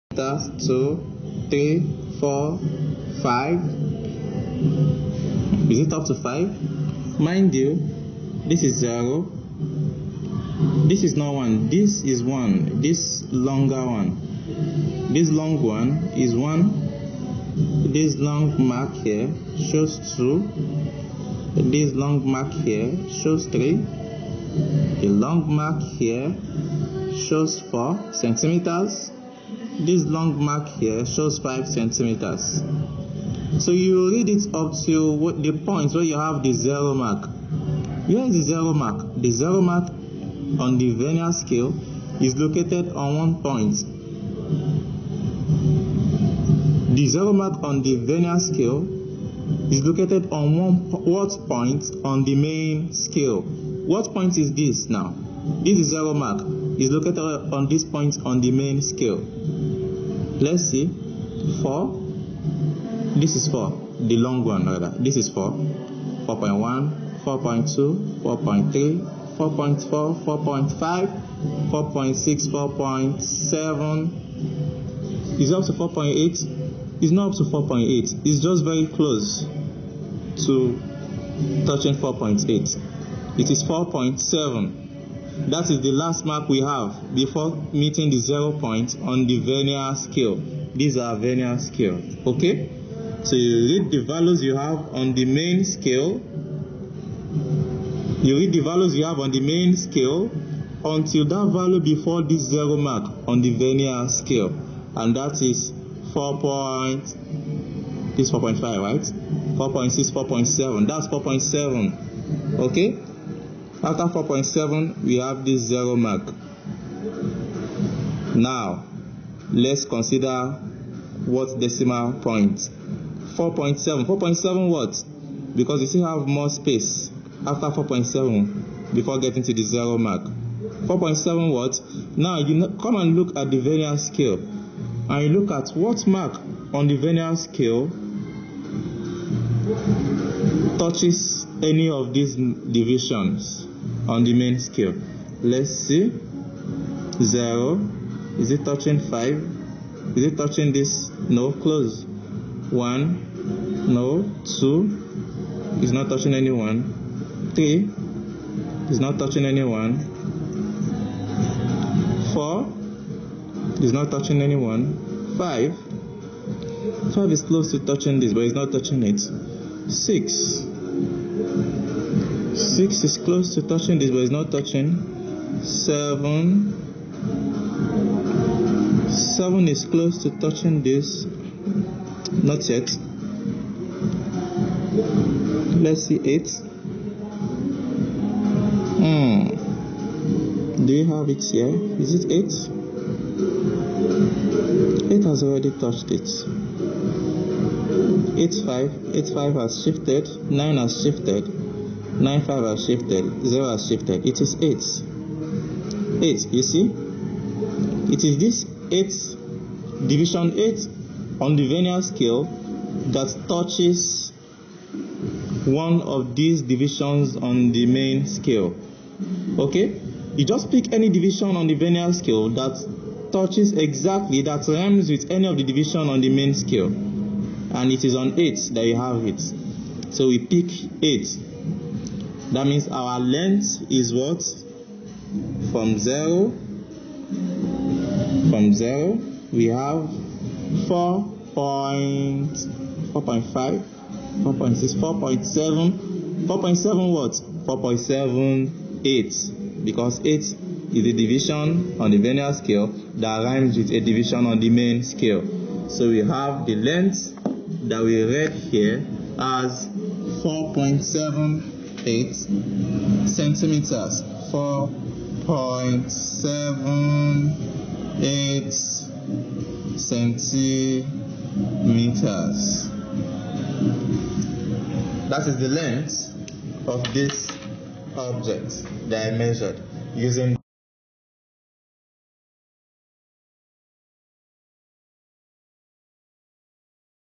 How to use a Vernier Calliper to measure the length of an object
This video explains how to use a Vernier Calliper to measure the length of an object while putting parallax error into consideration